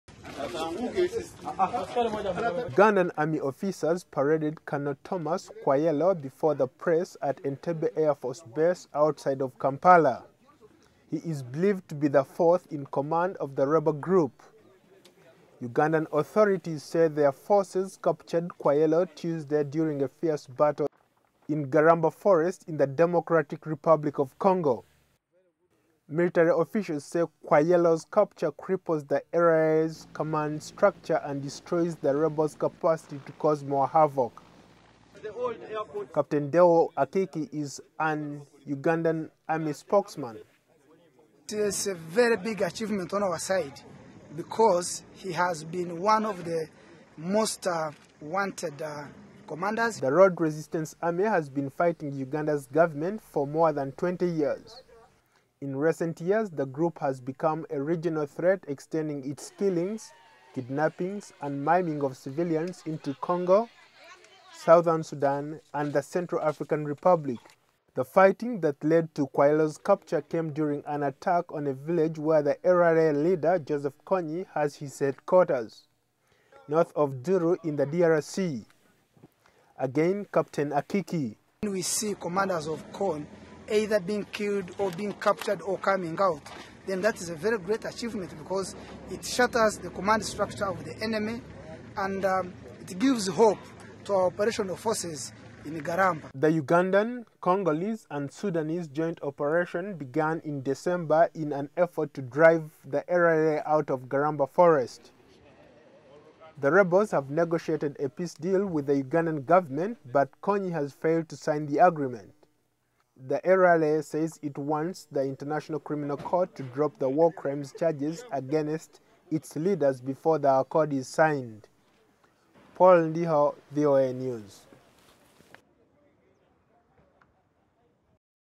Joseph Kony deputy Commander Captured
Ugandan Military officials say they have captured one of LRA's top commanders